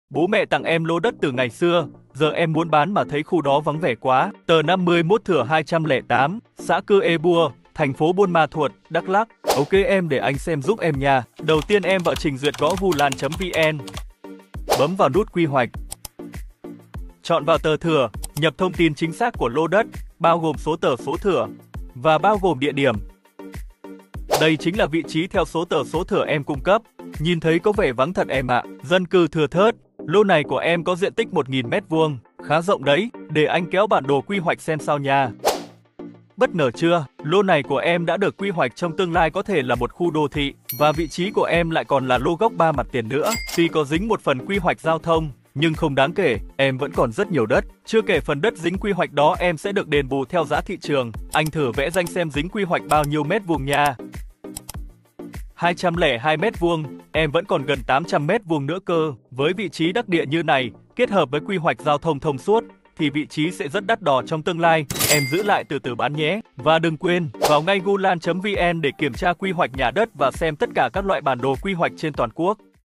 Check quy hoạch đất cùng Guland và hướng dẫn check quy hoạch
Việc **check quy hoạch đất** là bước vô cùng quan trọng trước khi mua bán hoặc xây dựng trên bất kỳ mảnh đất nào. Quy hoạch đất đai ảnh hưởng trực tiếp đến giá trị và tính pháp lý của bất động sản. Nếu không kiểm tra quy hoạch kỹ lưỡng, người mua có thể gặp phải rủi ro mua đất dính quy hoạch, không được phép xây dựng hoặc bị thu hồi mà không được đền bù hợp lý.

Hiện nay, việc tra cứu thông tin quy hoạch trở nên đơn giản và nhanh chóng hơn nhờ các công cụ trực tuyến như **Guland**. Guland giúp bạn tra cứu quy hoạch một cách chính xác, minh bạch và dễ dàng, cung cấp thông tin chi tiết về mảnh đất mà bạn quan tâm. Bạn có thể biết được mảnh đất có nằm trong khu quy hoạch giao thông, quy hoạch cây xanh, hay khu vực công trình công cộng hay không.

### Hướng dẫn check quy hoạch đất cùng Guland:
1. **Truy cập Guland**: Bạn chỉ cần truy cập vào trang web của Guland hoặc tải ứng dụng di động để bắt đầu.
2. **Nhập thông tin**: Sau đó, bạn nhập thông tin cụ thể của mảnh đất như địa chỉ, số thửa, hoặc tọa độ.
3. **Xem kết quả**: Guland sẽ hiển thị thông tin quy hoạch chi tiết, bao gồm mục đích sử dụng đất, kế hoạch quy hoạch, và các dự án xung quanh.

Việc kiểm tra quy hoạch sẽ giúp bạn tránh được những rủi ro không mong muốn và đảm bảo tính pháp lý khi mua bán hoặc xây dựng nhà cửa. Sử dụng **Guland** để an tâm trong mỗi quyết định đầu tư bất động sản!